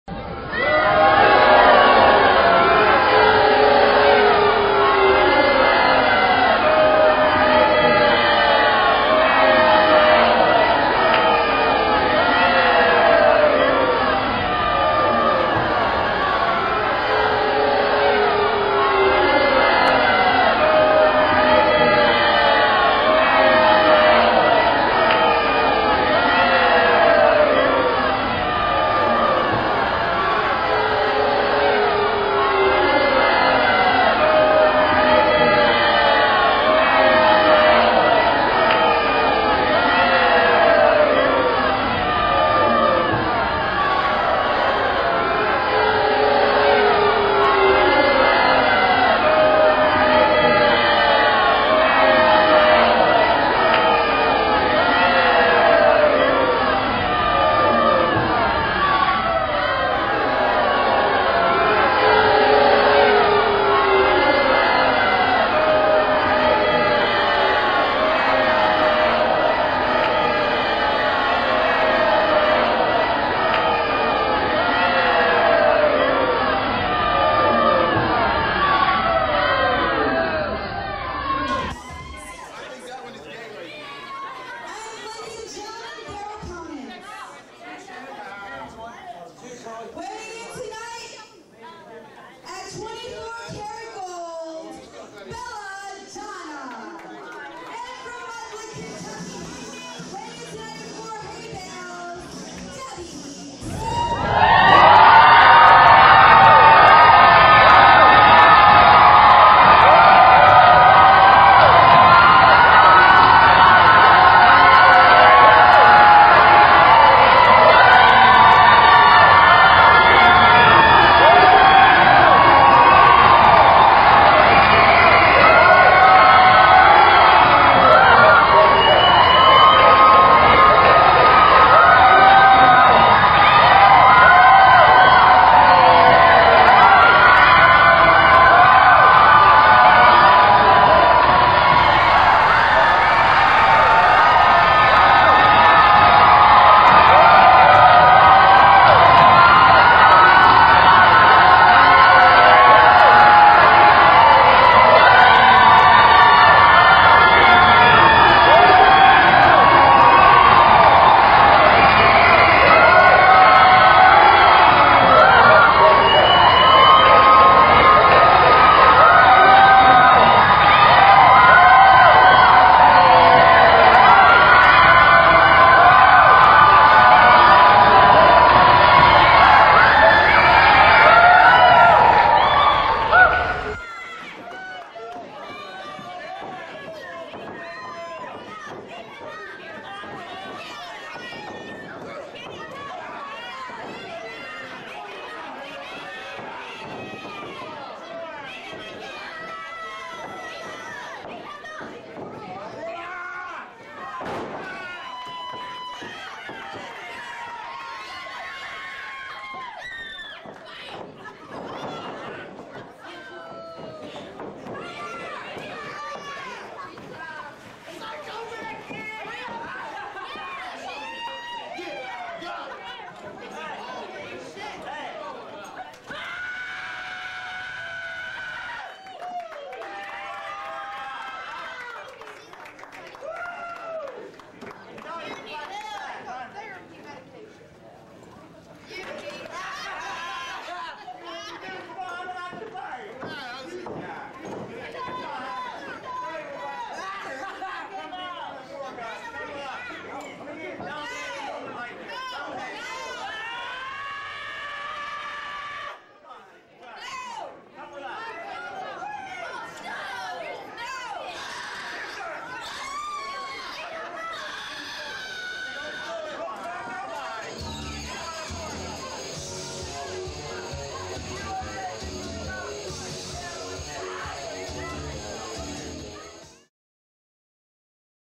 WPW | Debbie Sue and Belladawna vs. Autumn Breeze and Kylie Pierce (November 2, 2007)
From WPW's Flamingo Cup 2007, Autumn Breeze and Kylie Pierce take on Belladawna and Debbie Sue!